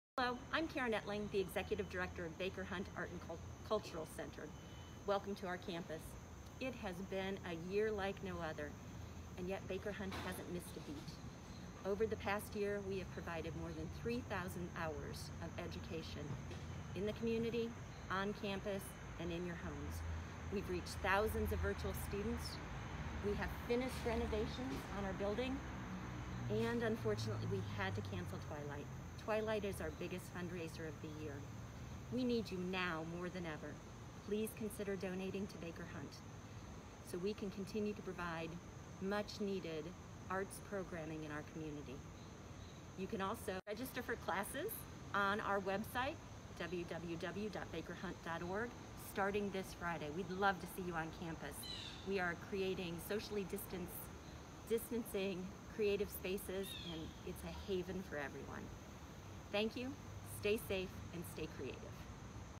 Announcements from Baker Hunt
We've been busy in this season of quarantine, social distance, and just a year like no other. Tune in for some important announcements from Karen Etling, our Executive Director. Find out more about our campus for information on classes and ways to donate.